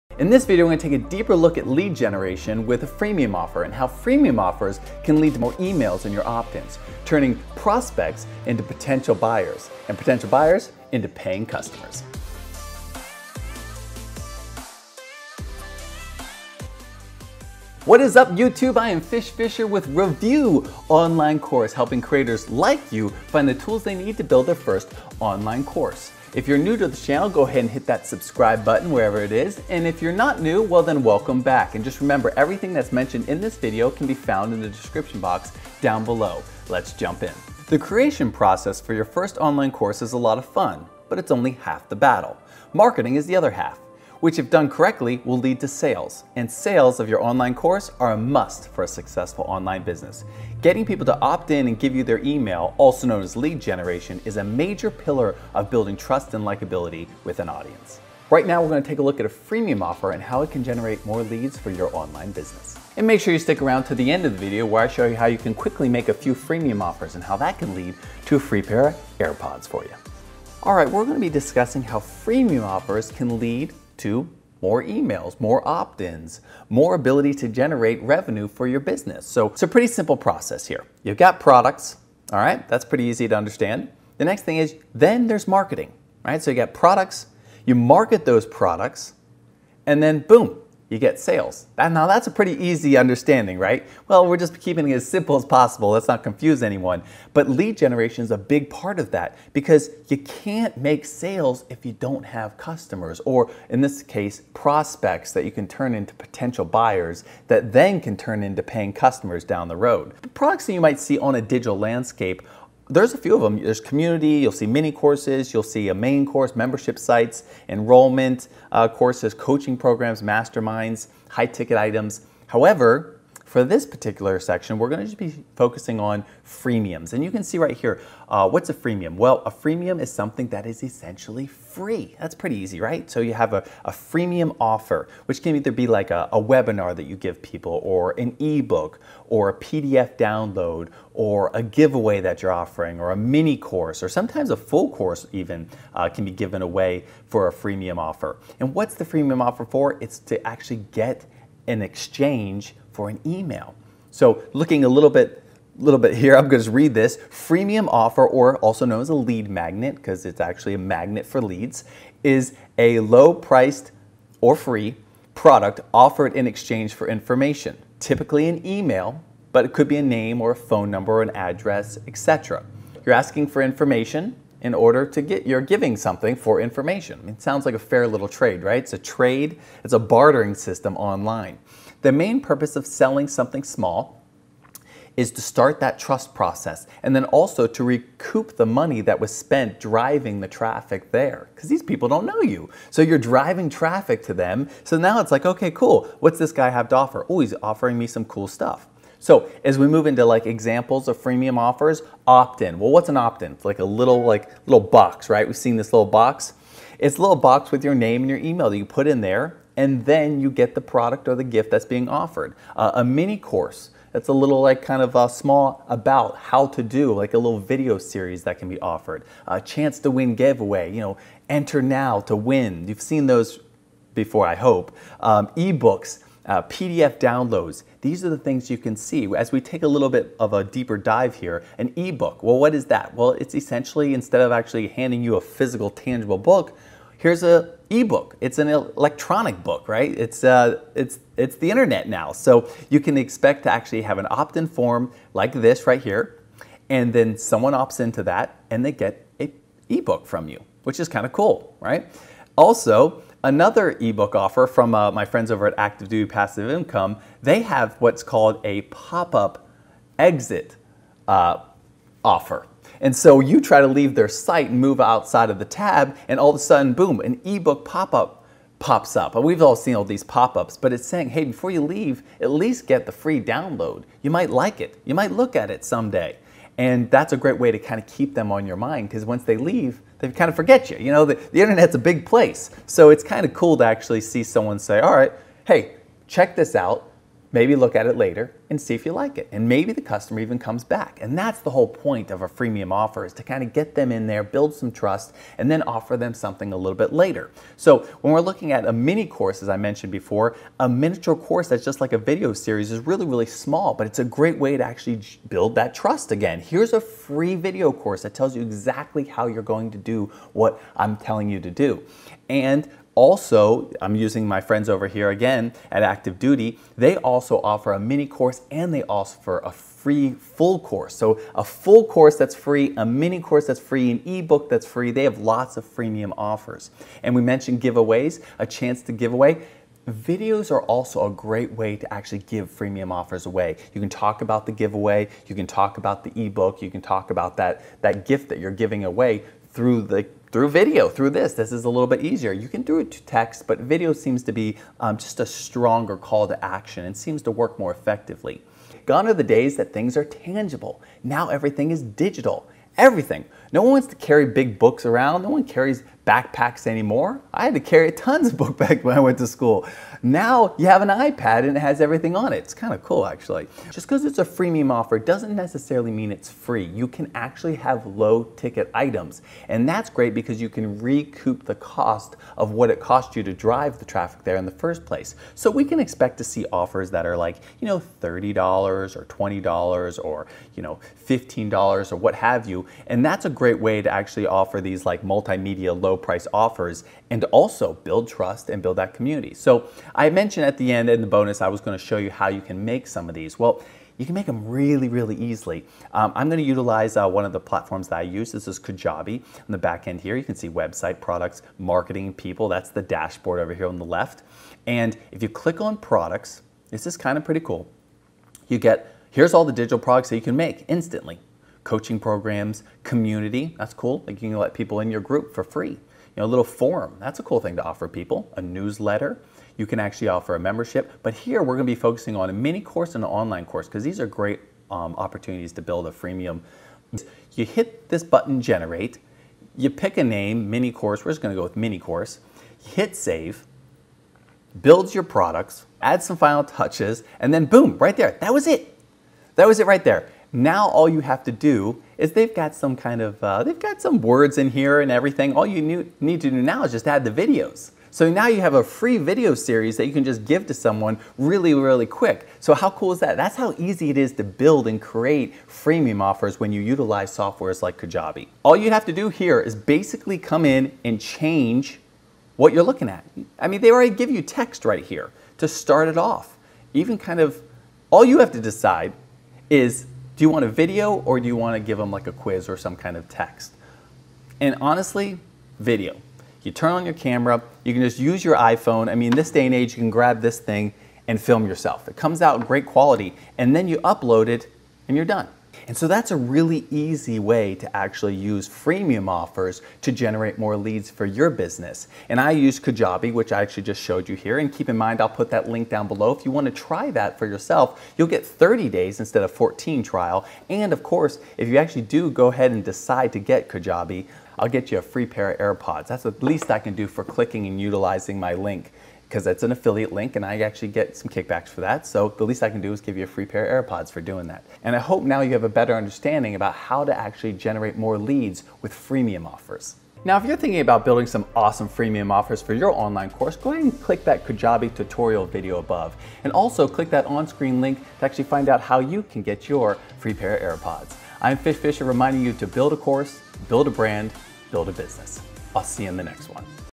Lead Generation Through Freemium Offers
Utilizing a freemium lead generation model may reduce the final cost required to earn new customers or clients. This happens through shifting the educational burden from marketing and sales teams to the actual end user.

As a two tiered lead generation system, freemium models often divide users by free tier or paid tiered. Users belonging to the free tier will experience reduced features or access while using the product, while paid tiered users have greater access to the full array of features available.

The ultimate goal of lead generation through freemium offers revolves around reducing the overall cost for customer acquisitions (customer acquisition cost or CAC). Many freemium models eliminate the need to pay for opening an account, which will clearly lower the commitment needed to start experiencing some of the features offered by your product.

Freemium models often reduce the spending that companies dedicate to traditional paid marketing outlets or to their overall sales processes. Freemium offers create the trust and provide the value that companies typically rely on highly trained (and paid) sales teams to convey when engaging leads.

By allowing a potential buyer to experience a free version of your product or service, you're providing value and building trust in the process. Many proponents say that the freemium period allows for natural lead generation as users learn about the value and power in your products by using them before buying.

After achieving success with your product, free tier users will eventually reach the limits of the free-level account and decide to invest in a premium account in order to gain access to the full set of features and value that your product provides.

Freemium offers are usually controlled through a variety of ways, which include feature limitations.

Other examples of freemium offers include ebooks, mini courses, full online courses, PDF forms and video lessons. These offers have proven to provide interested consumers with information and value about what your products or services provide and quite often lead to a sale.